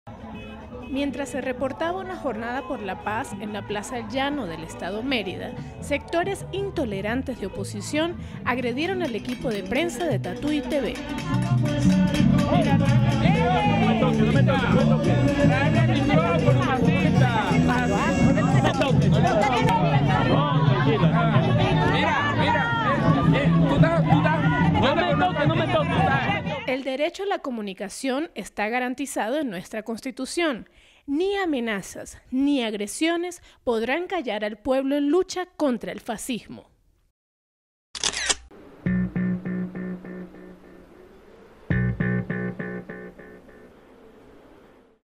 Agredido equipo de Prensa de Tatuy Tv en Mérida
Mientras se reportaba una jornada de paz en la Plaza El Llano del Estado Mérida, sectores intolerantes de oposición agredieron al equipo de Prensa de Tatuy Tv.
El derecho a la comunicación está garantizado en la Constitución de la República Bolivariana de Venezuela. Ni amenazas, ni agresiones podrán callar al pueblo en lucha contra el fascismo.